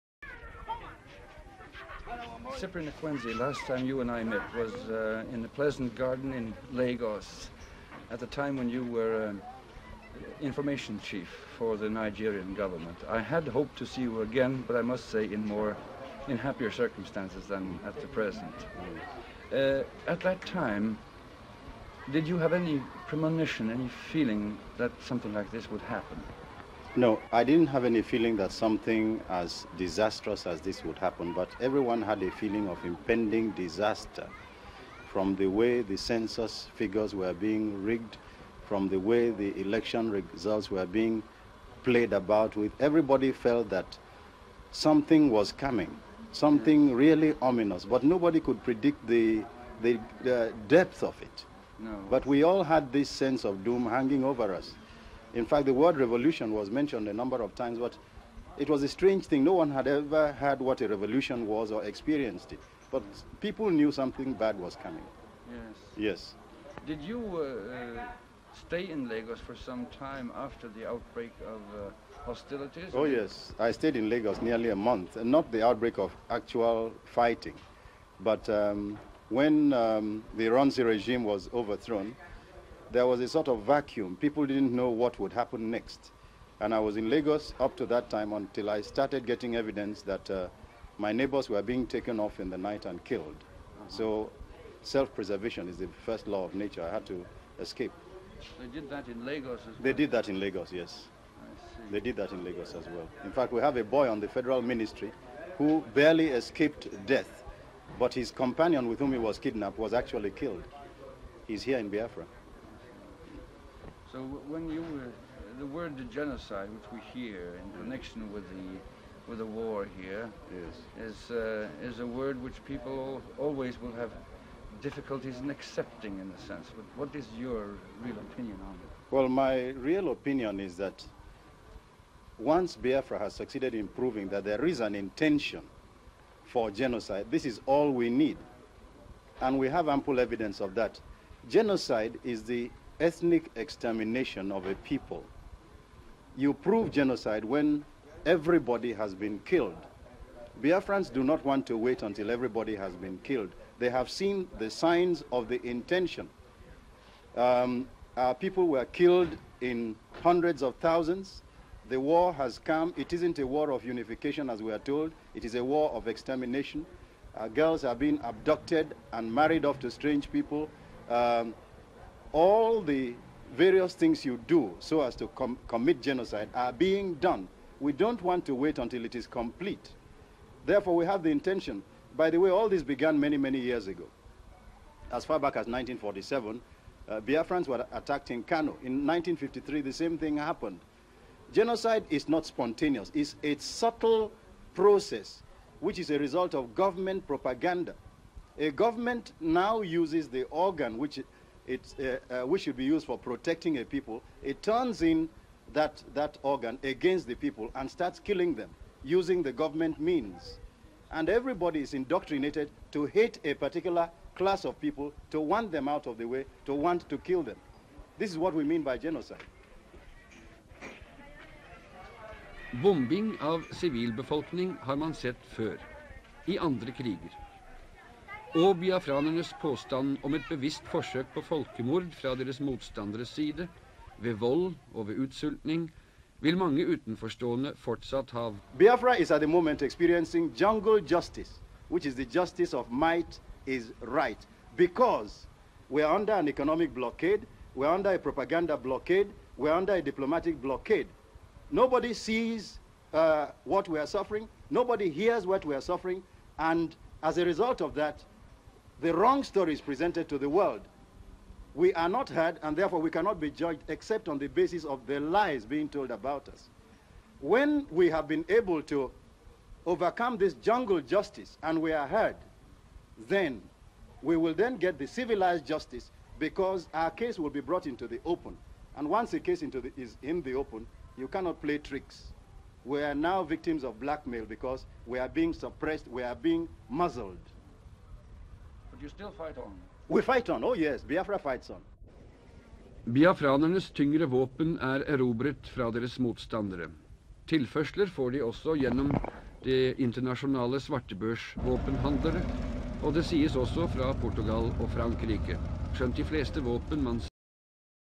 This Day in 1969: Cyprain Ekwensi speaks on Biafran war. This is a must watch! | August 27, 1979.
Biafran Spokesman, Cyprain Odiatu Ekwensi speaks on Biafran war. He was interviewed in Enugu by BBC's Richard James.

"It was a war of extermination and not unification as we were told..." - Ekwensi told the press

Cyprain Ekwensi was also a Pharmacist, Broadcaster, Veteran writer who wrote top classics such as Nana Jagua, Passport of Mallam illa, African Nights Entertainment, Samankwe And The Highway Robbers, etc.